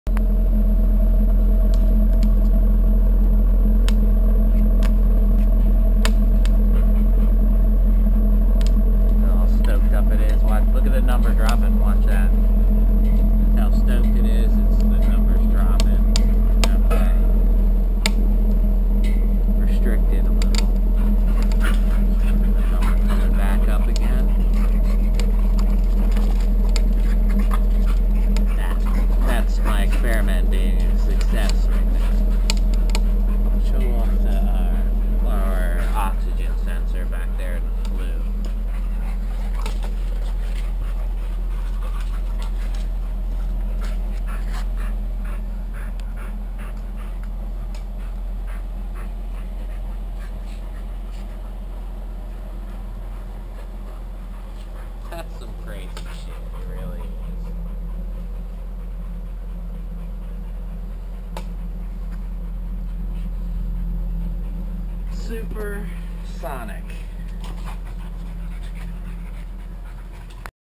rocket heater with narrow band o2
This is the angry bird rocket heater with a o2 bung welded in the flue pipe and a narrow band o2 to try the idea before I spend 60 bucks on a wideband sensor . When I make changes to the air in take the voltage output of the o2 reacts . This o2 sensor is no where near the right range for a fuel with stoich AFR of 7.5 but it worked good enough to try the concept .